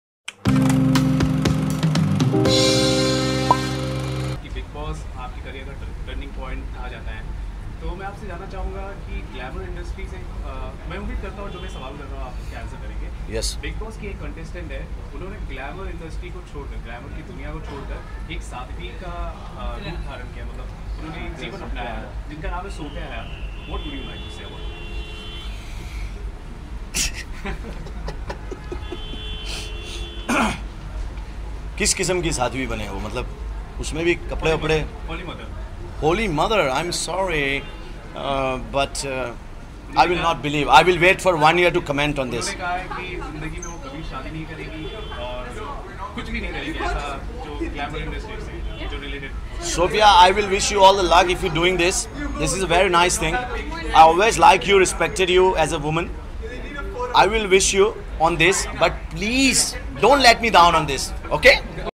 Sofia Hayat please don't let me down: Ajaz Khan, watch video | Filmibeat
Former Big Boss contestant Sofia Hayat turned into nun and reacting to that her former co-contestant Ajaz Khan finds hard to believe this. Watch out what Ajaz Khan had to say about Sofia Hayat. Watch video here.